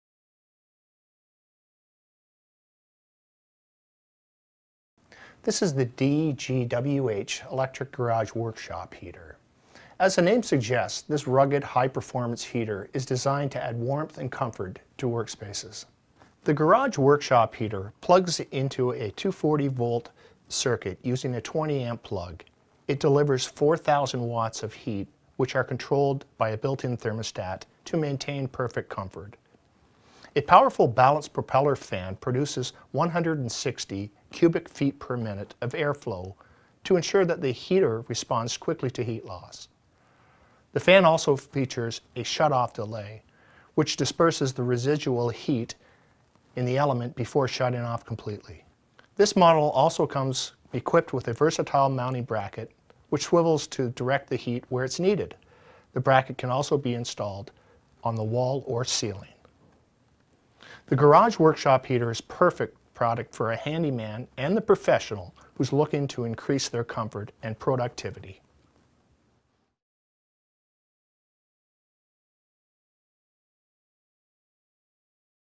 Dimplex Garage Workshop Heater
Dimplex Garage Workshop Heater provides warmth and comfort to work spaces.  With a versatile mounting bracket, heat can be directed where it is needed.  For all your heating needs, Construction Solutions and Supply is here to help you choose the right heater for your work area.